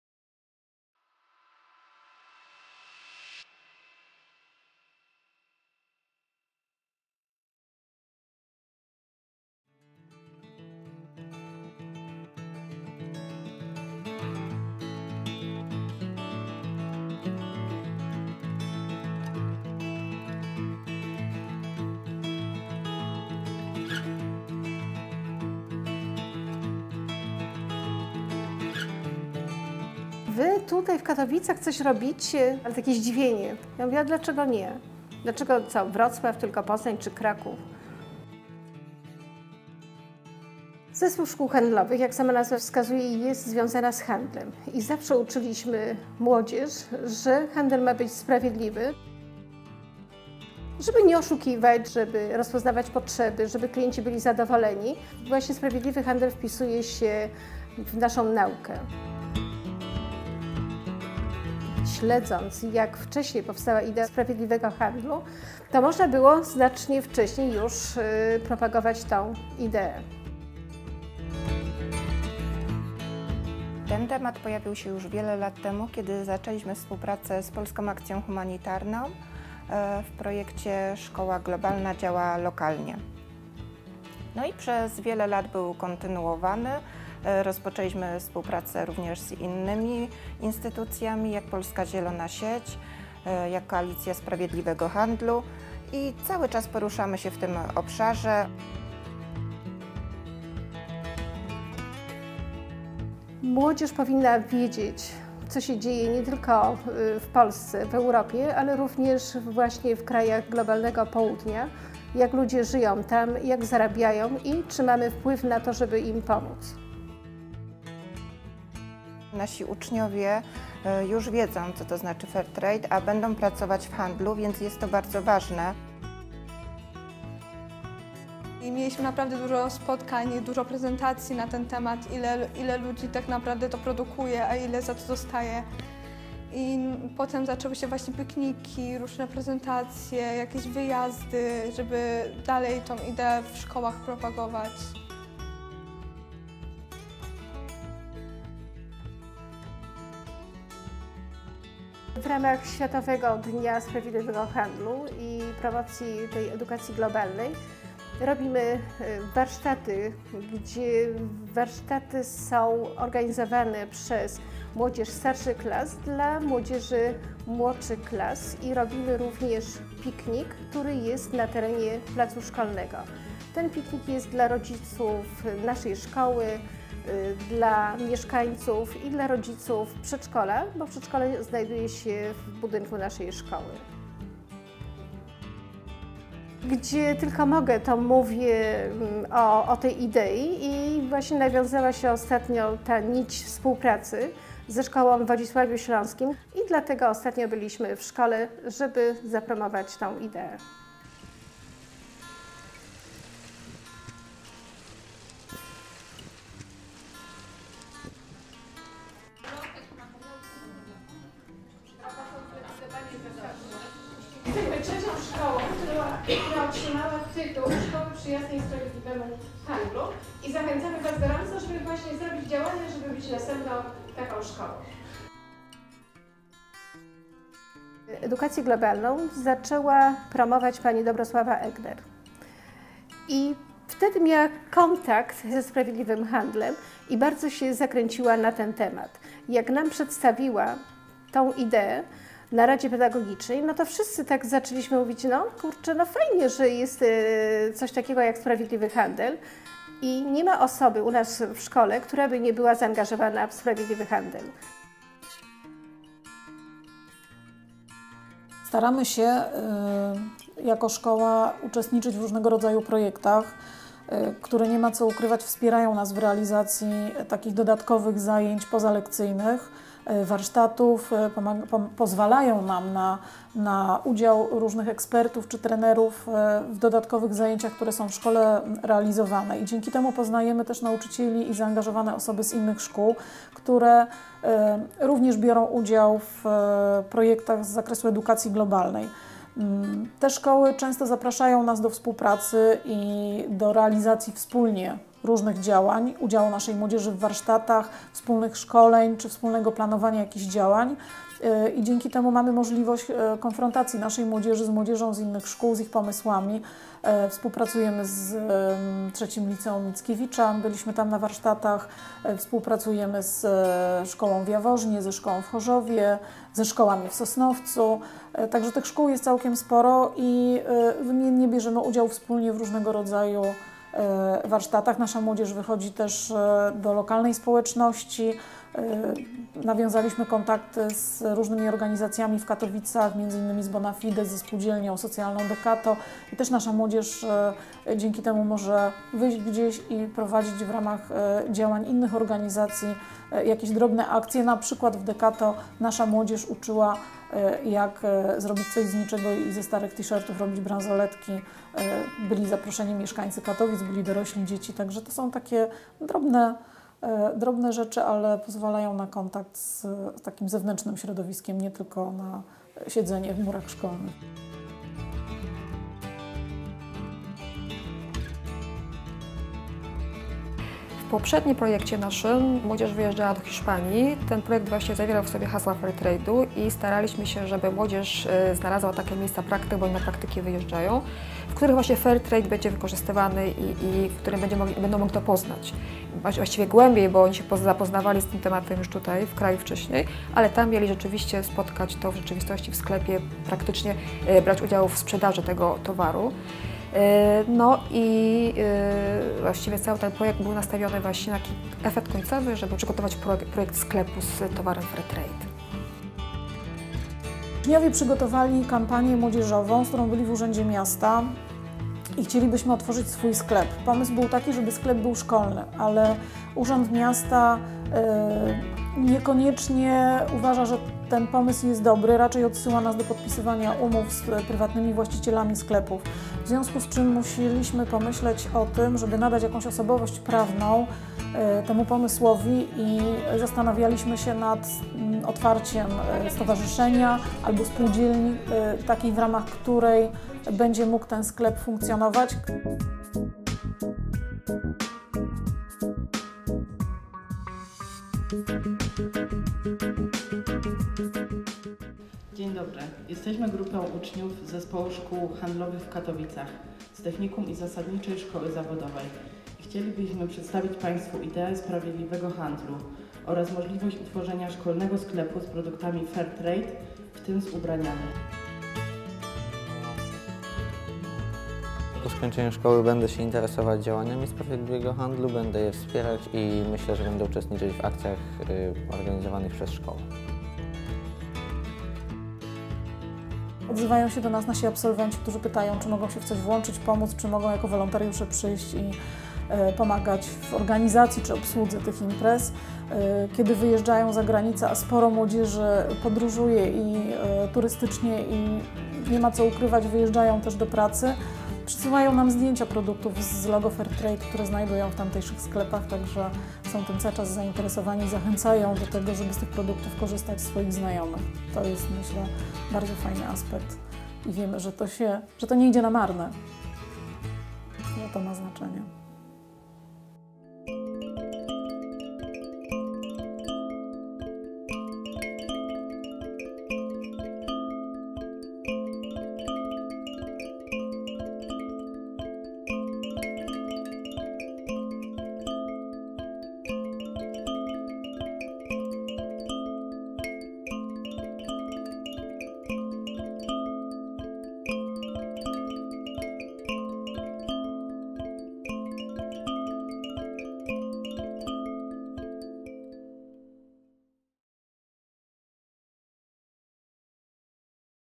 Boleslaw Prus Commerce Schools Complex in Katowice, Poland (Fairtrade Polska)
This video by Fair Trade Polska presents the Boleslaw Prus Commerce Schools Complex
in Katowice, a trade school complex actively engaging in promoting Fair Trade. The school regularly organises Fair Trade events in the town where it is located (Katowice). It is one of the first schools in Poland to have received the Fair Trade School title.
The European project SUSY is presenting examples of Good Practices in Social and Solidarity Economy around the world. Find out more: solidarityeconomy.eu